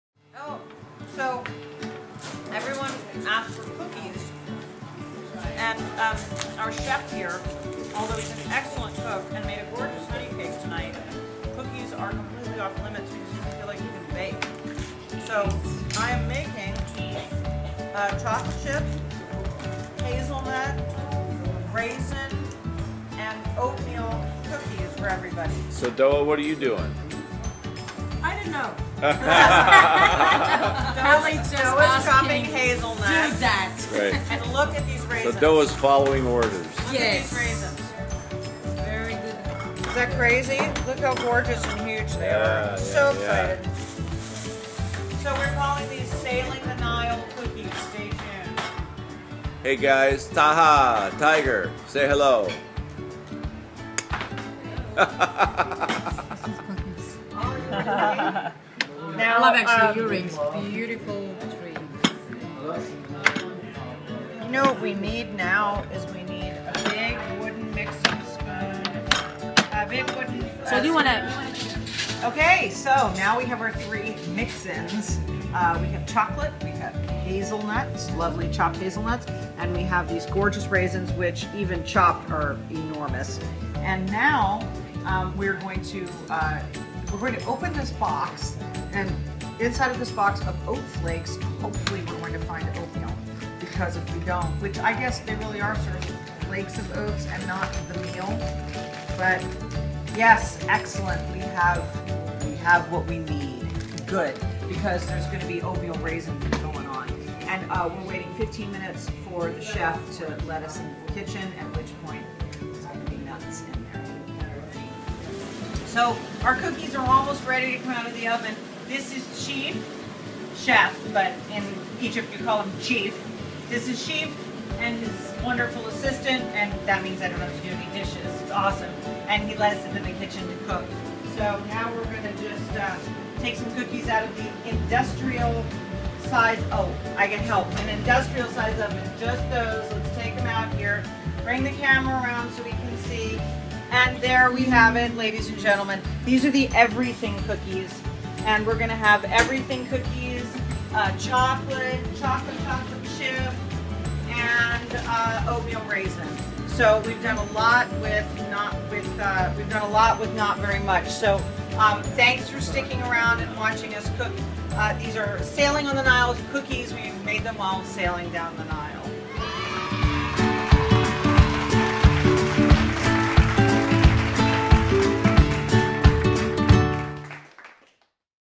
"Sailing the Nile" Cookies
Everyone on "Egypt 2012: The Dawning" was craving some homemade cookies, so after a trip to the market in Luxor for supplies, we whipped a batch (of about 8 dozen) of special "Sailing the Nile" cookies!